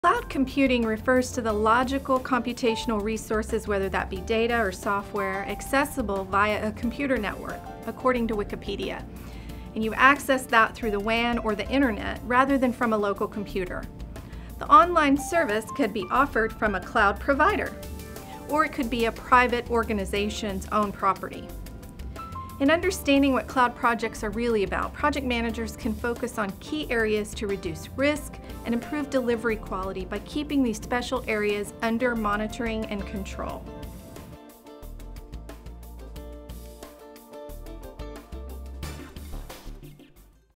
Meet the Speakers – Tresia Eaves | IPM Day 2015
IPM Day 2016 registration is open!

Stay Calm with the Cloud.

Meet Tresia Eaves, Principal Consultant of Variant Enterprises. For IPMDAY 2015, her presentation was on the topic of “Project Managers and Cloud Computing.”

It’s Cloud projects versus other kinds of projects. Project Managers often face challenges with multiple vendors or organizations pulling and tugging on resources, technology interfacing differences, and many other quality challenges they would not otherwise face.

This presentation shared techniques to help you guarantee delivery success on schedule, budget and with all the features you were expecting!